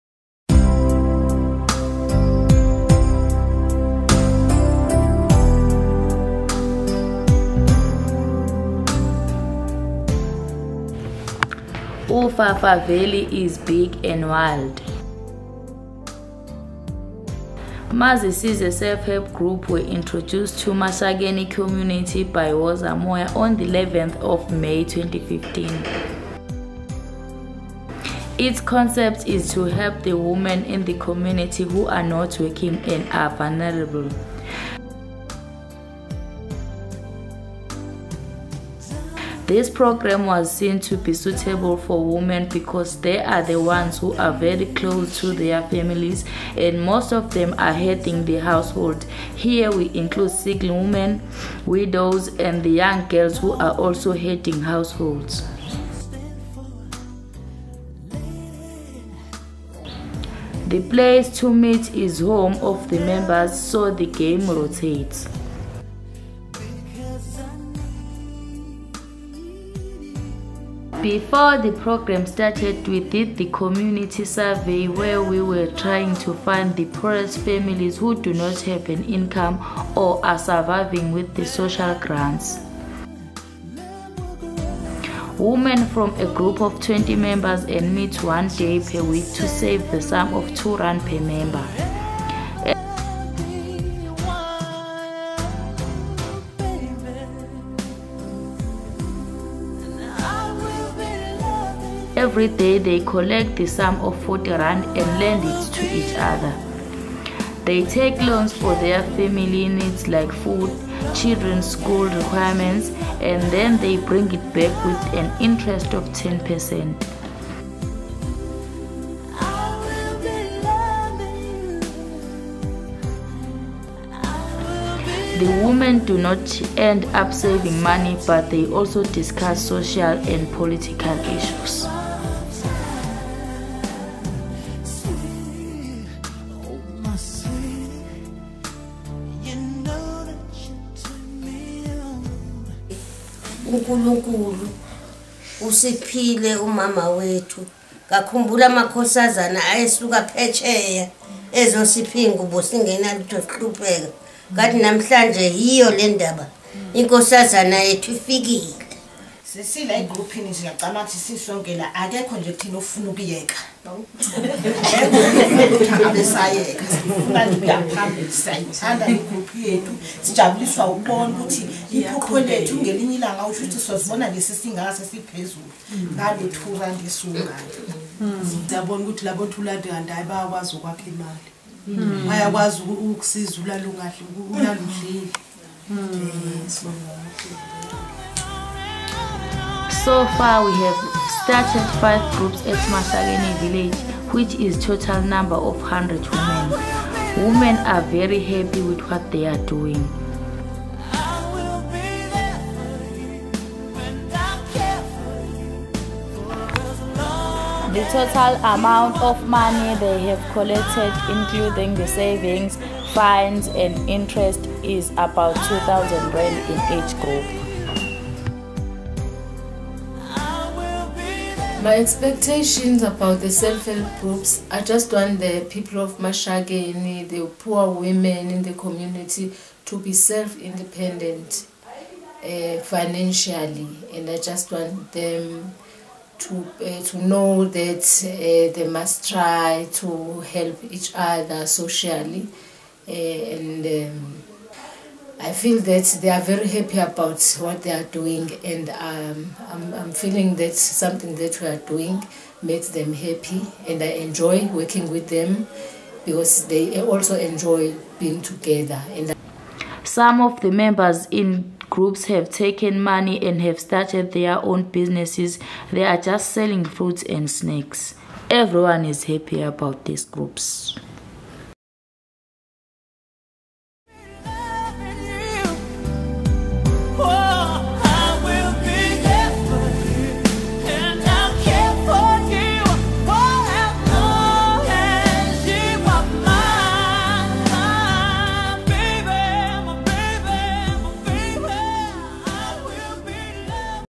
Mazisize Self Help Group
Established on 11 May 2015 a group of vulnerable women save R2 a week. By lending the money out again, with interest, they are able to empower and support themselves.